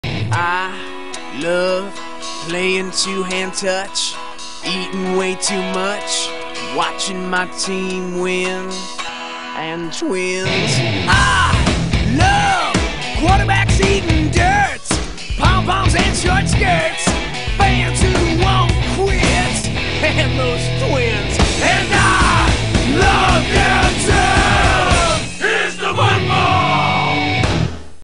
Here's to Football
Coors Light’s most successful commercial campaign of all time (2002)